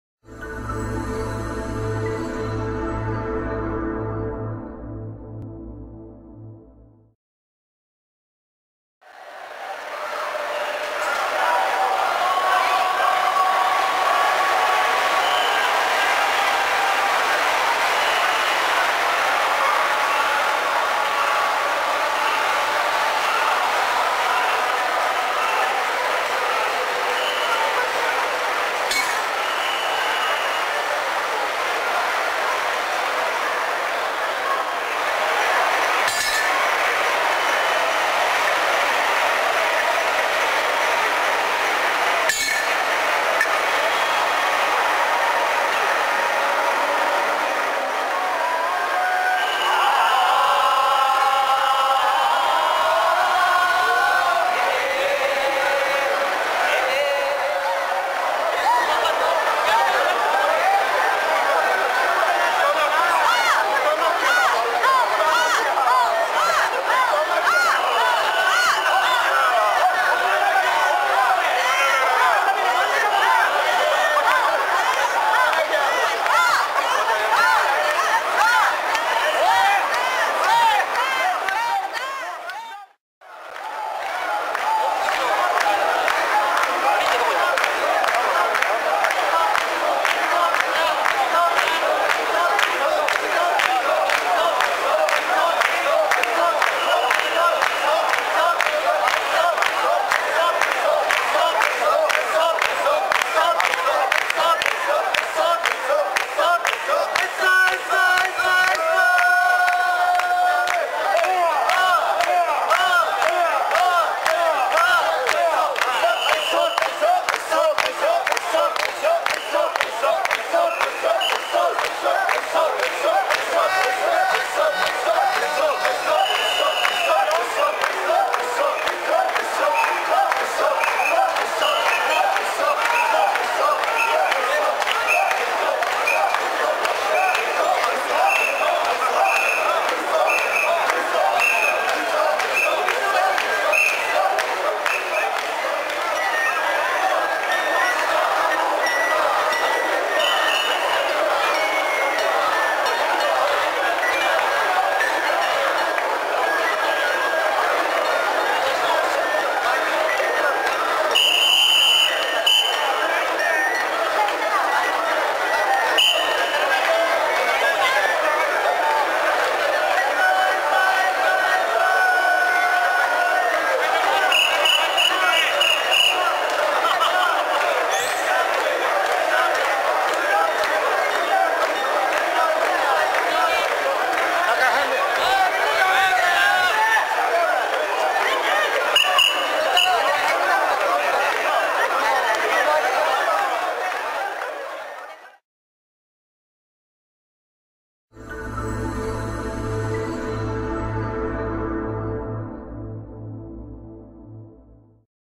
平成24年 練馬・白山神社 例大祭 本社神輿渡御＝千川通り
有数の欅（けやき）の巨木です。永保三年（1083年）、源義家が「後三年の役」で奥州に向かう際、戦勝を祈願してこの社に奉納されたものと伝わります。
手古舞と鉄棒そして鳶頭と神職が明治神輿を先導する。台輪三尺五寸（106ｃｍ）　他に子供神輿等4基も巡行する。　宮入し境内での神輿揉みは花棒争いから小競り合いの中の着輿。
2012年10月14日